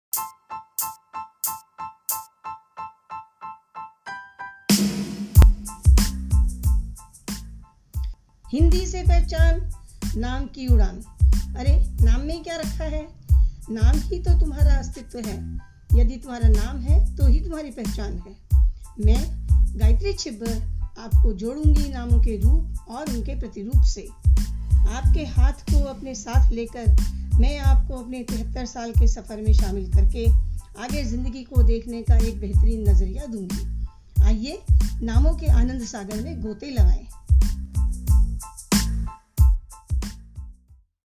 Trailer
Today we mark a grand start towards a common goal of imparting knowledge with this podcast.
You can find this on your favorite podcast streaming service.
Voice: Gayitri Chhibber
Production: Puranjay Savar Mattas
Editing Software (Audio): Audacity
Editing Software (Video): Adobe AfterEffects